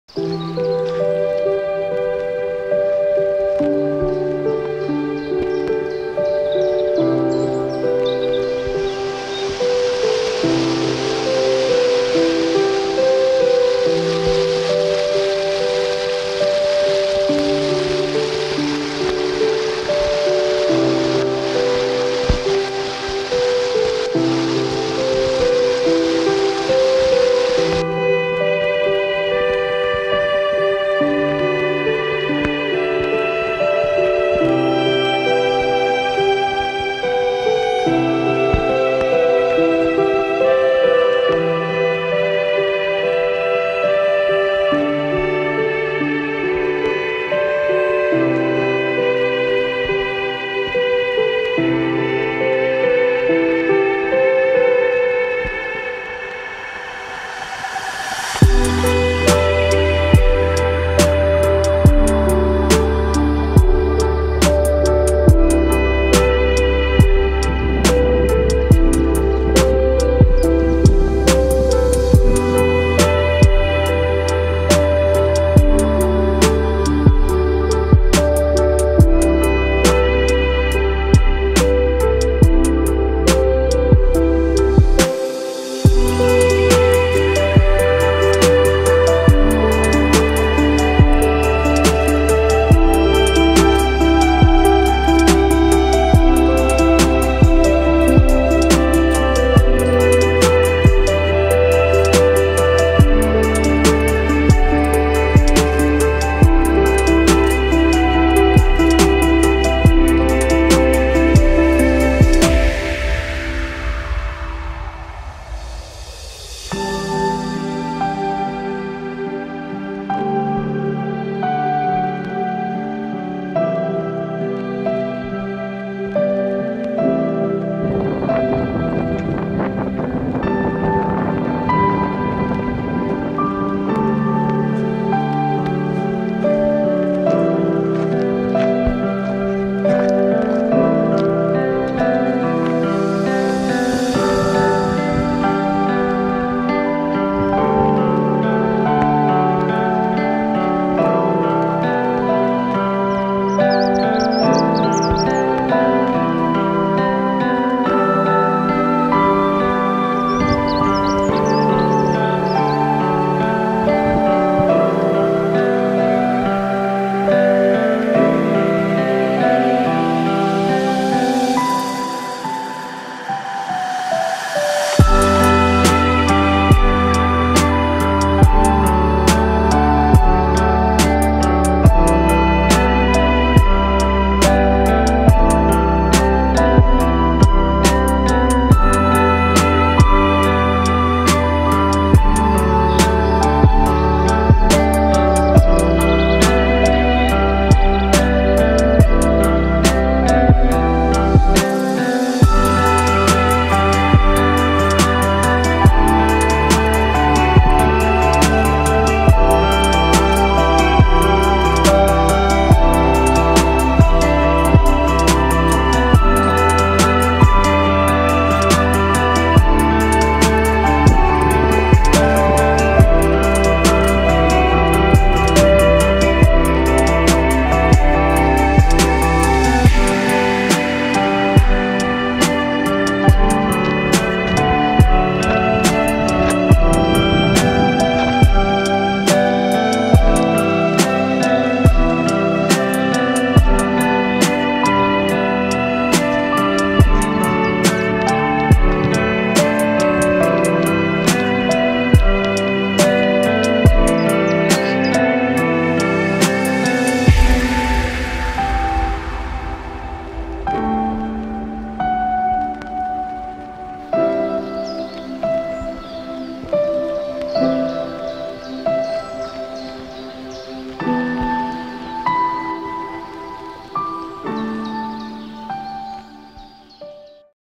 в Горганах...
Відео з походу за маршрутом: Осмолода - гора Коретвина - гора Попадя - гора мала Попадя - гора Паренки - полонина Плісце - гора Грофа - Осмолода